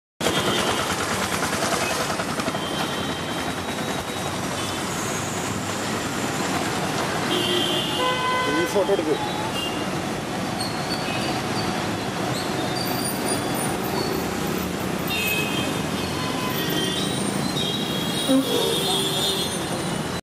VOLVO B11R i-SHIFT KALLADA TRAVELS
Volva B11R i-shift belonging to Kallada Travels spotted at Kollam Kerala .
The B11R provides first-class driveability, outstanding fuel-efficiency and low environmental impact.  For reliability, safety and long-term profitability, the B11R has been designed for long service intervals and simple maintenance extending its product life cycle.

Volvo I-Shift is a fast-acting gearchanging system with brief interruptions in torque and a wide ratio span, resulting in ample resources for high average speeds. The gear lever is integrated into the driver's seat and gear-changes are entirely automatic. The starting gear is selected automatically according to the road's gradient and vehicle load.